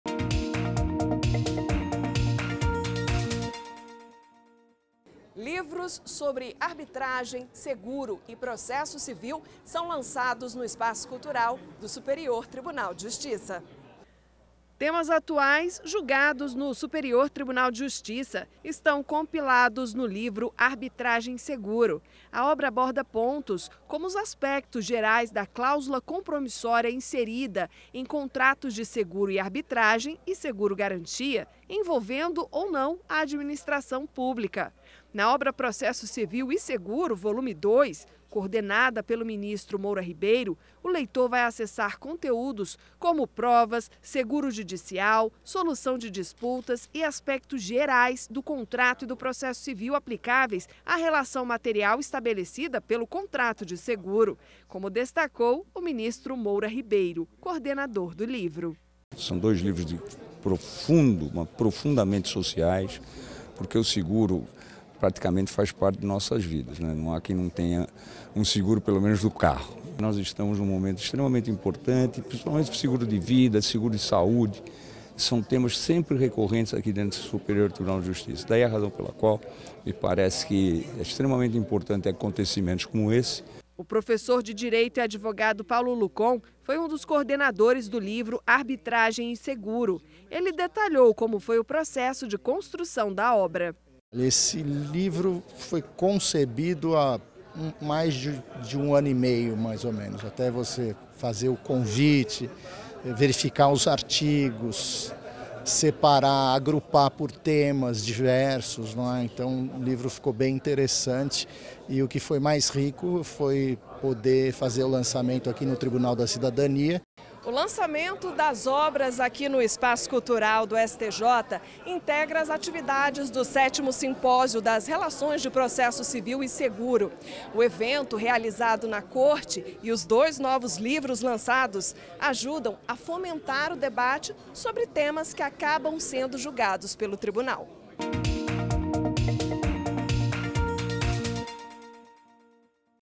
Após simpósio, livros sobre seguro são lançados no Espaço Cultural do STJ
​Na noite desta terça-feira (12), o Espaço Cultural do Superior Tribunal de Justiça (STJ) sediou o lançamento dos livros Arbitragem & Seguro e Processo Civil e Seguro - Volume II.  A primeira obra é coordenada pelo ministro Ricardo Villas Bôas Cueva e conta com a participação dos especialistas Paulo Henrique dos Santos Lucon, Cássio Telles Ferreira Netto, Luís Antônio Giampaulo Sarro, Luís Fernando Crestana e Vivien Lys Porto Ferreira da Silva.
Já a obra sobre processo civil e seguro tem como coordenadores o ministro Moura Ribeiro e os juristas Paulo Henrique dos Santos Lucon, Luís Antônio Giampaulo Sarro e Luís Fernando Crestana.